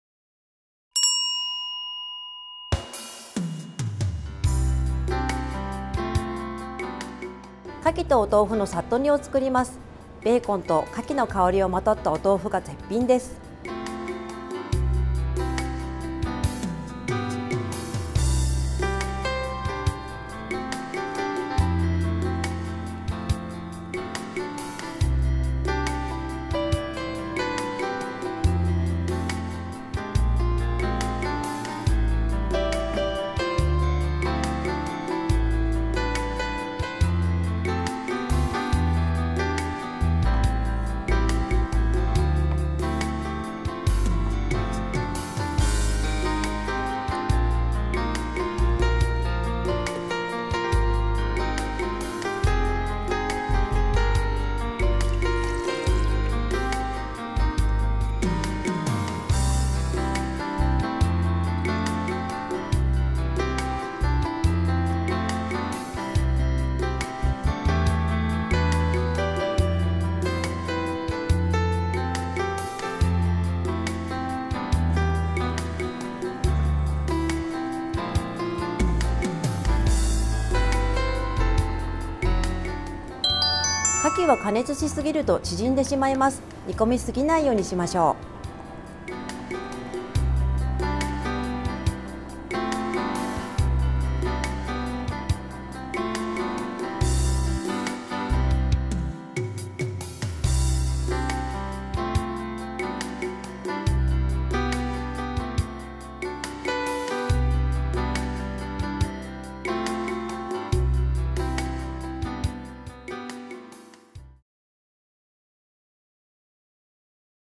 カキと豆腐のさっと煮［1人分185kcal］［西川満希子先生］
カキは加熱しすぎないように！

カキむき身　…8粒
ベーコン　　…1枚
木綿豆腐　　…1/2丁
片栗粉　　　…大さじ1
春菊　　　　…1株
白髪ねぎ　　…適量
ゆずの皮　　…適量

(A)
カツオだし汁…1カップ
しょうゆ　　…大さじ1と1/2
みりん　　　…大さじ1と1/2

◇作り方
1) カキは洗って水気をふき取り、豆腐は６等分にし、片栗粉を薄くまぶす。ベーコンは1cm幅に切り、春菊は食べやすい大きさに切る。
2) 鍋に(A)を合わせ、豆腐とベーコンを入れて火にかけ、沸騰したらカキと春菊を加え火を通す。
3) 器に(2)を盛りつけ、白髪ねぎとゆずの皮を添える。